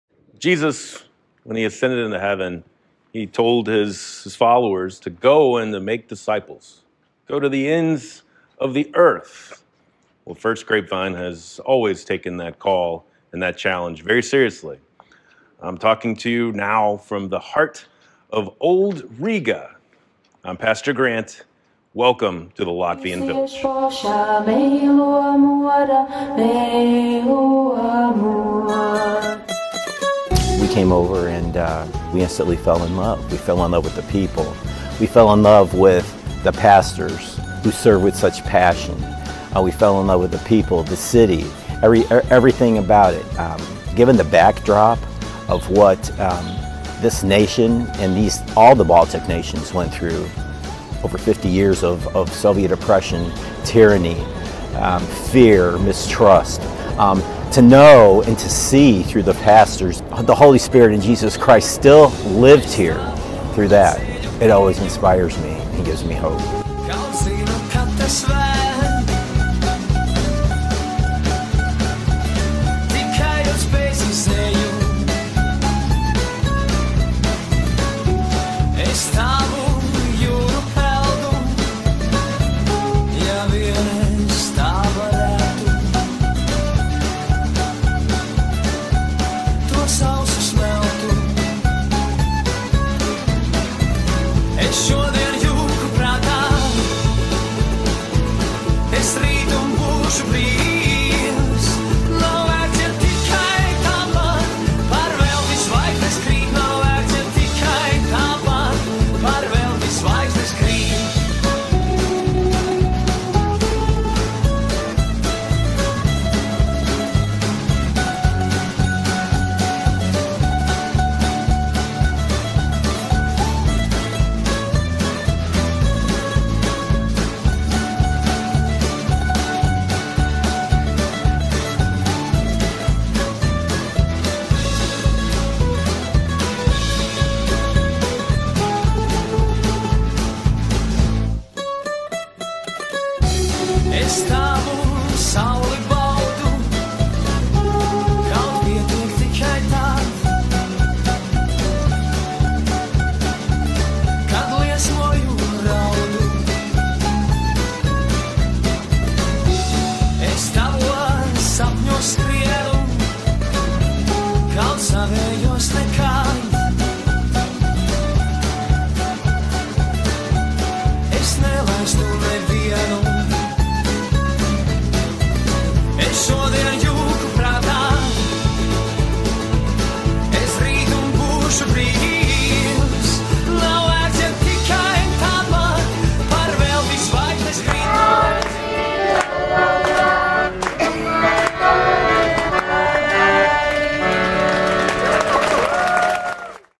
Introduction to Latvia 2024
First Grapevine Mission Team's Trip to Latvia in June 2024